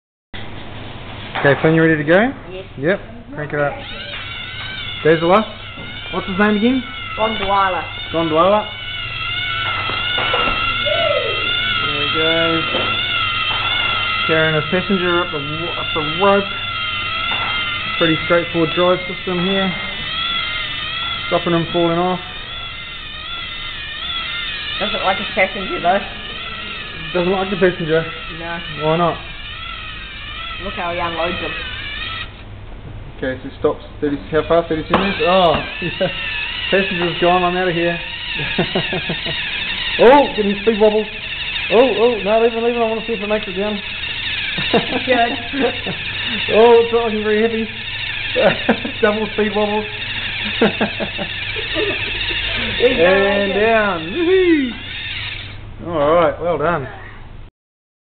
Greg Scrivin Lego NXT Climbing 'gondola' robot
The design brief for this challenge was to have your robot start on the ground carrying a ball as a 'passenger', then climb a string up towards a wall, stop before the wall to let the passenger off, then descend back down to the bottom.  The students built a variety of designs, this one just uses big rubber wheels to get some good speed. Well done Finn - good robot design.